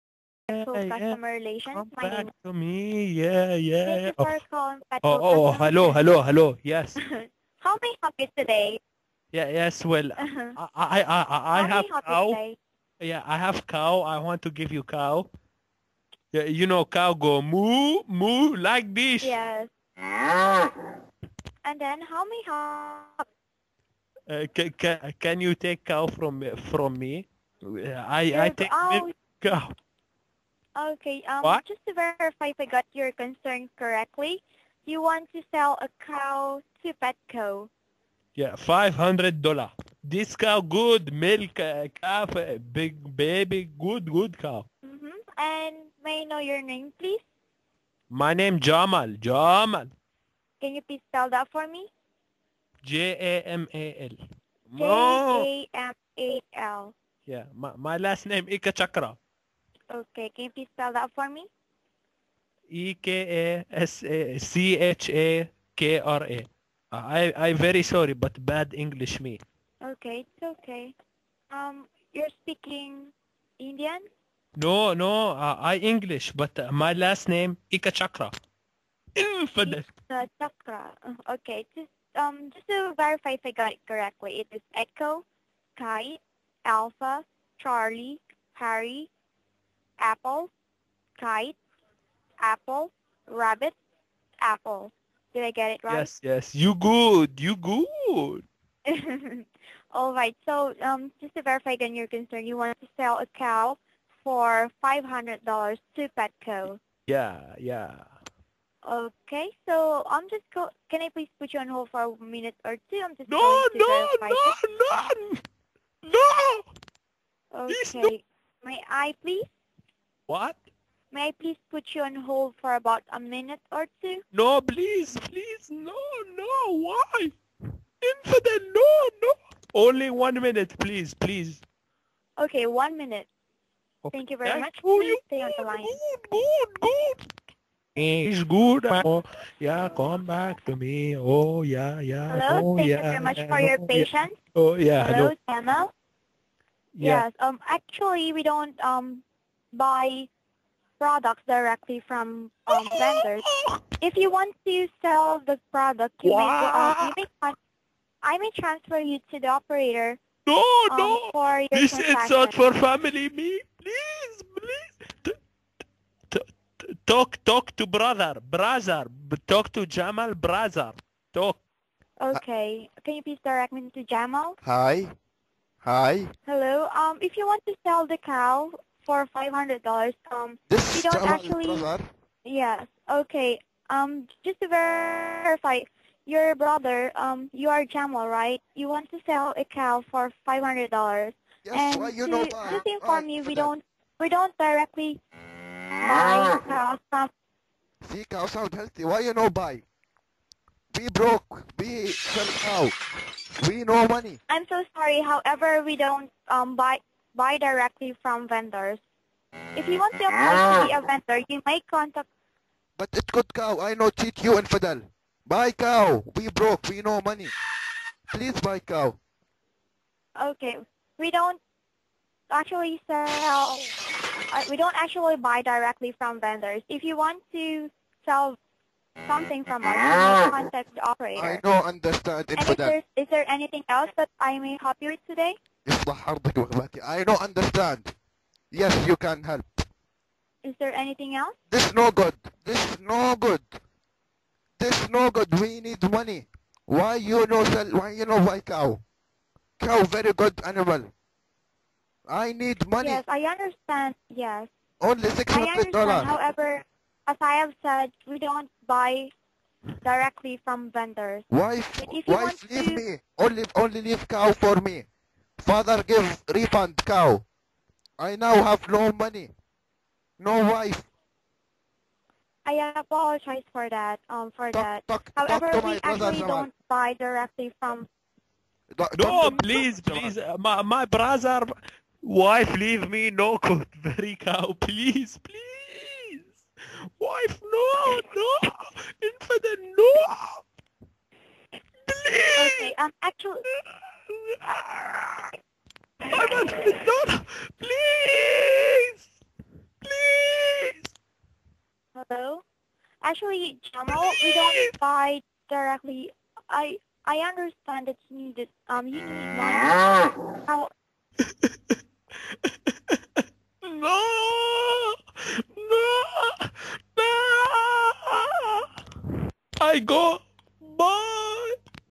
Prank call! Selling a cow to Petco! (Hilarious indian accent)
Just me any my friend trying to sell a cow on Petco ;)